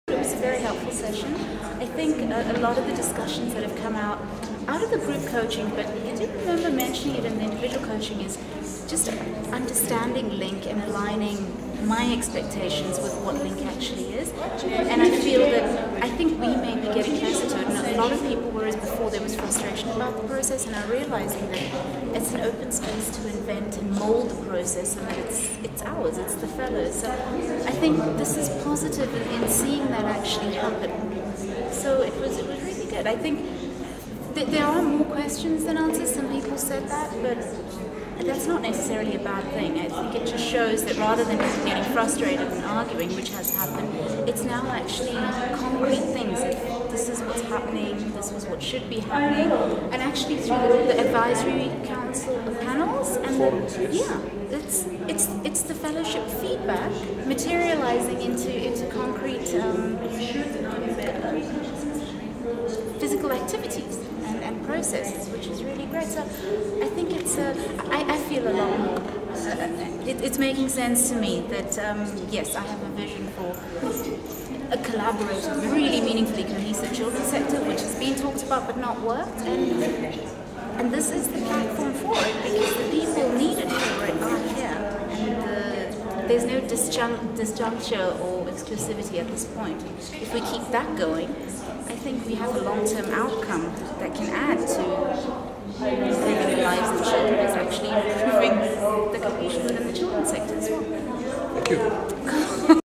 Maria Kurian Feedback.MOD
A LINC Fellow giving feedback on the Consultation meetings held in KZN, Gauteng and Western Cape for the LINC Fellowship